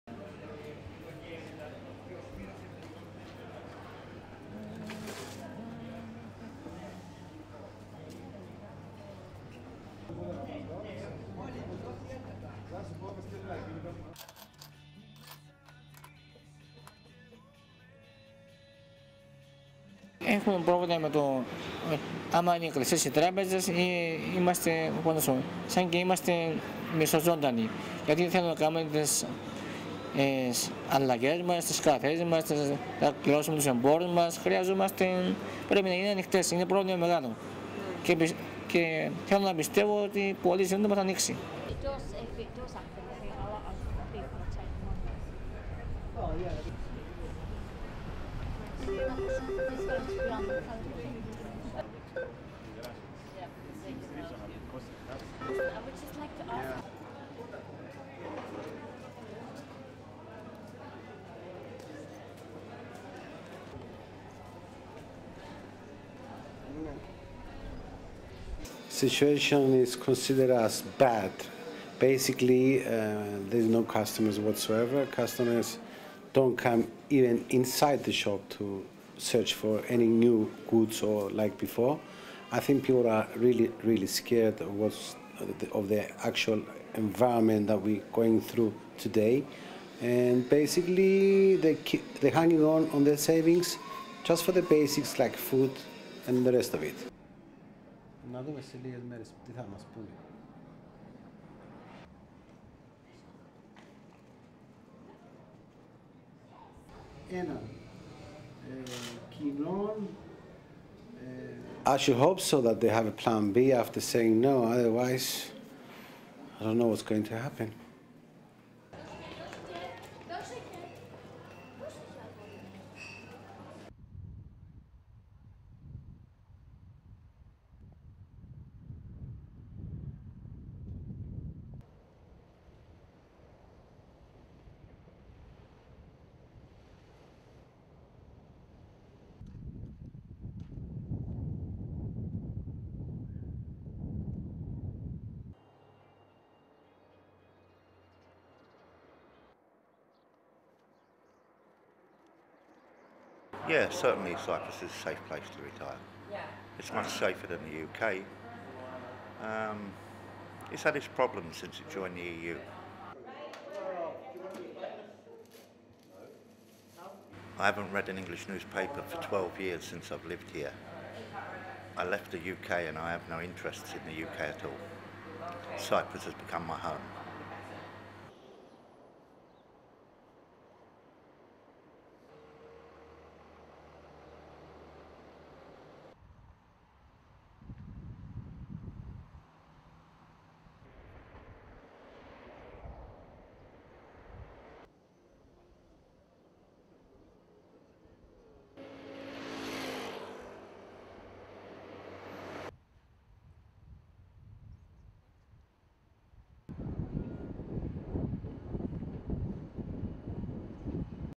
Business owners, UK ex-pats struggle to pay bills as banks remain closed
Nicosia, Cyprus
1. Wide exterior of flower shop in Nicosia shopping district of Ledras, florist, Stelios Stylianou, picks up a tray of flowers
2. Close up of flowers outside shop
3. Mid of Stylianou looking through an empty cash register
4. SOUNDBITE (Greek) Stelios Stylianou, Florist:
"We have a problem when the banks are closed. It's like we are the living dead, we can't make any withdrawls, we can't make deposits, pay our suppliers. They (the banks) must be open, because its causing a huge problem. I want to believe that they will open soon."
5. Wide of people queuing to take money out of cash machine
6. Mid of people queign at cash machine
7. Close up of card being taken out of cash machine after declined transaction
8. Wide of passerby looking at window and goods for sale then walking away
9. Mid of shoe store owner in downtown Totis Pelacanos, Nicosia walking anxiously outside of his shop.
10. SOUNDBITE (English) Totis Pelacanos, shoe shop owner in downtown Nicosia:
"The situation is considered as bad. Basically there are no customers what so ever, there aren't even any customers inside the shop to search for any new goods or like before, I think people are really really scared of the actually environment we are going through, today, and basically, they are hanging onto their savings, just for the basics like food and the rest of it."
11. Mid sof Totis Pelacanos, shop owner
12. Close up cutaway of face of Totis
13. Close up cutaway of shoes inside of shop
14. SOUNDBITE (English) Totis Pelacanos, Shoe shop owner in downtown Nicosia:
"I should hope so that they have a plan B after saying no otherwise, I don't know what's going to happen�."
15. Close up of sign in shop window, reads (English and Greek): "Open"
Limassol, Cyprus
16. Various of Limassol Molos Promenade
17. Mid of people on Limassol beach
18. Wide of fisherman's boat in water off Limassol beach
19. Wide exterior of 'Taste of Britain Delicatessen'
20. Close up of sign outside shop, reads (English): "Taste of Britain: Deli and Coffee Shop"
21. SOUNDBITE (English) Richard Savage, British expatriate shop owner:
"Yeah, certainly Cyprus is a safe place to retire. It's much safer than the UK. It's had its problems since its joined the EU."
22.  Mid of Richard Savage, British expatriate shop owner, inside Taste of Britain serving a customer
23. SOUNDBITE (English) Richard Savage, British expatriate shop owner:
"I haven't read an English newspaper for twelve years. Since I've lived here. I left the UK and I have no interest in the UK at all. Cyprus has become my home."
24. Wide of a branch of 'Laiki Bank'
25. Mid of people queuing to get the money out of the cash machine
26. Wide of a cafe in Limassol old port area
27. Wide of Limassol sea front
28. Various of people sitting in a sea front cafe
29. Mid of cargo ship anchored off shore, palm tree in foreground
30. Wide top shot of Limassol Marina construction site, Akrotiri peninsula in the background
STORYLINE
Cyprus' banks will remain closed Thursday and Friday as officials try to find a new plan to stave off bankruptcy.
The cash-strapped lenders have been closed since Saturday to avoid a bank run.
Cypriot officials rushed to find new ways to stave off financial ruin on Wednesday, including asking Russia for help, after Parliament rejected a plan to contribute to the nation's bailout package by seizing people's bank savings.
The bank's closure is having a direct effect on business.
"It's like we are the living dead," says florist Stelios Stylianou.